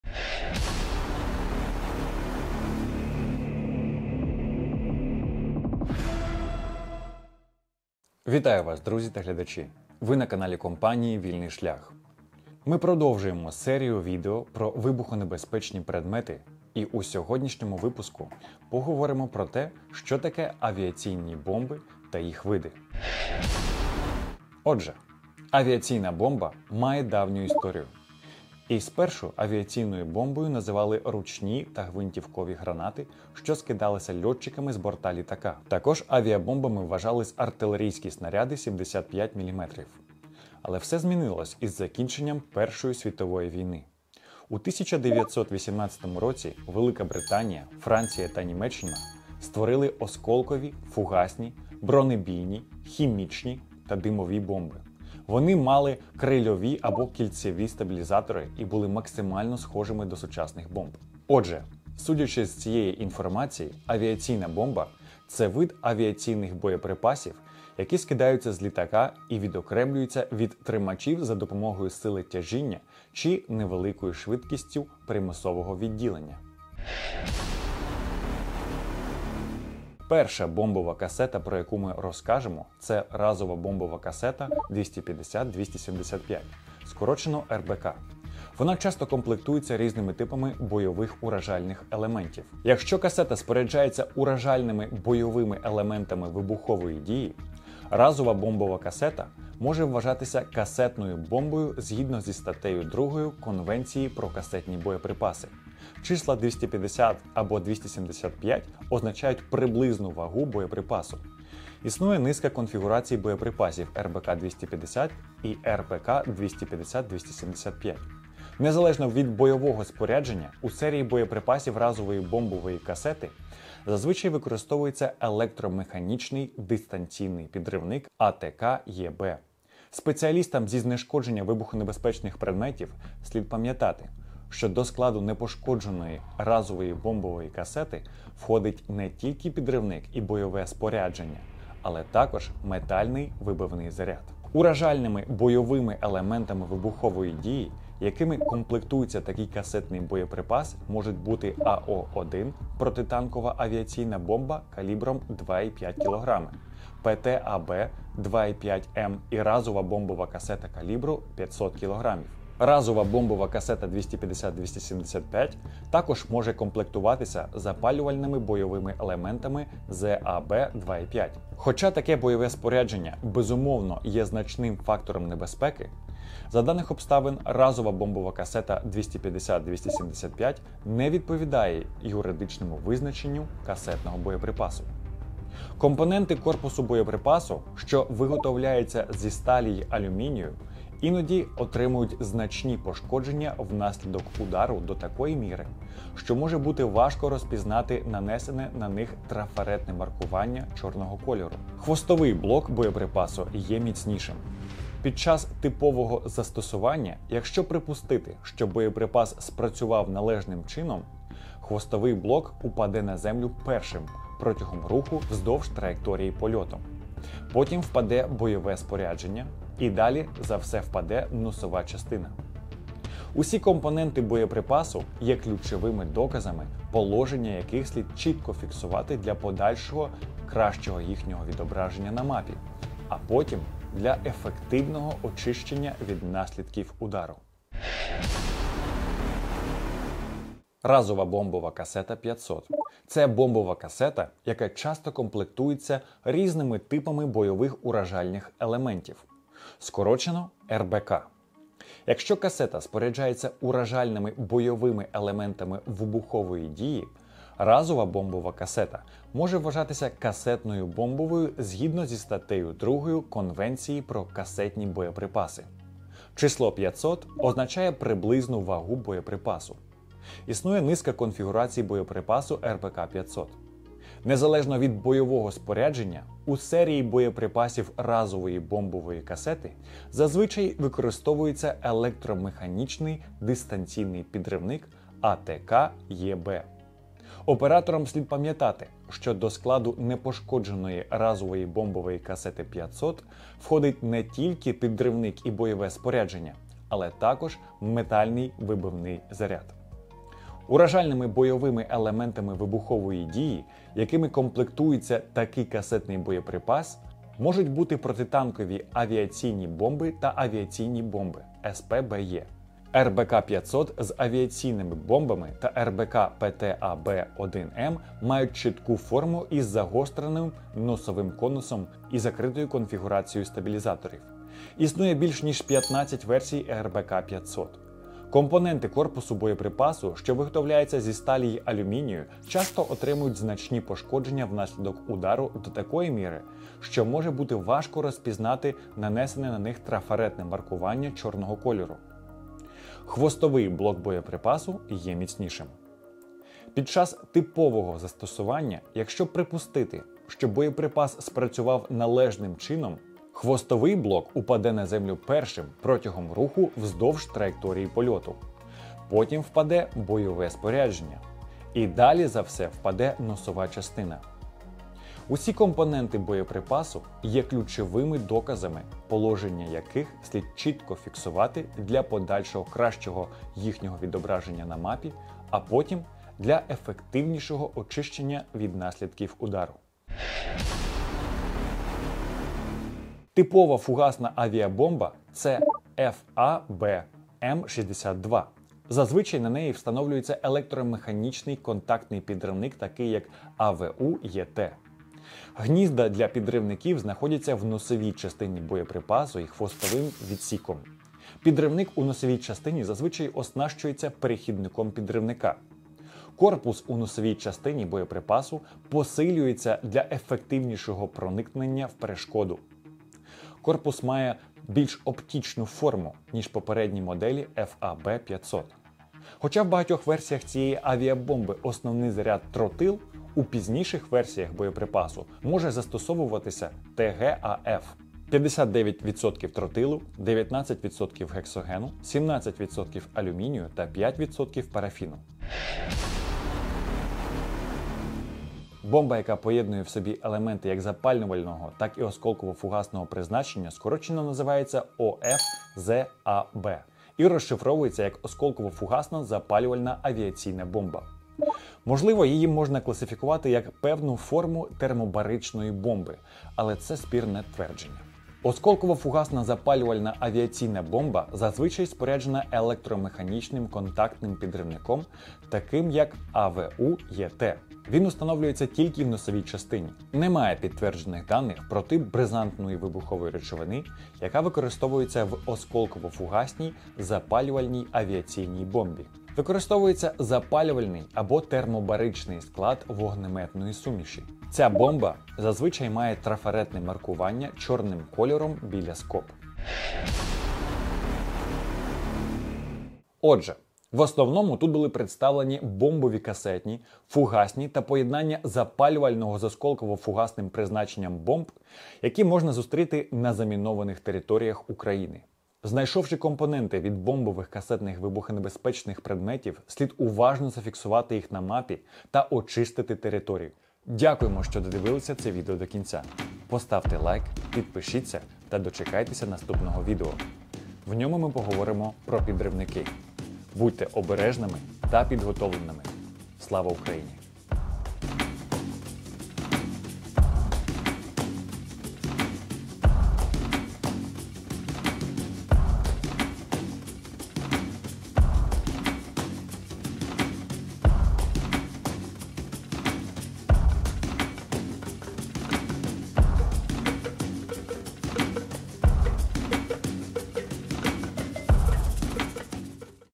Авіаційна бомба. Та види авіабомб.
Авіаційна бомба має давню історію. І спершу авіаційною бомбою називали ручні та гвинтівкові гранати, що скидалася льотчиками з борта літака. Також авіабомбами вважались артилерійські снаряди 75 мм. Але все змінилось із закінченням Першої Світової війни. У 1918 році Велика Британія, Франція та Німеччина створили осколкові, фугасні, бронебійні, хімічні та димові бомби. Вони мали крильові або кільцеві стабілізатори і були максимально схожими до сучасних бомб. Отже, судячи з цієї інформації авіаційна бомба – це вид авіаційних боєприпасів, які скидаються з літака і відокремлюються від тримачів за допомогою сили тяжіння чи невеликою швидкістю примусового відділення.

Ми продовжуємо серію відео про вибухонебезпечні предмети і у сьогоднішньому випуску поговоримо про те, що таке авіаційні бомби та їх види. Нагадаємо, що створення цих відео має мету навчити ідентифікувати небезпечні вибухові предмети спеціалістів з протимінної діяльності. Підготовка до цього випуску здійснена за матеріалами посібника "Вибухові боєприпаси", який підготував Женевський міжнародний центр з гуманітарного розмінування.

В основному тут були представлені :
- бомбові касетні
- фугасні
- та поєднання запалювального з осколково-фугасним призначенням бомб

Знайшовши компоненти від бомбових касетних вибухонебезпечних предметів слід уважно зафіксувати їх на мапі та очистити територію.

🇺🇦🇺🇦🇺🇦 Слава Україні!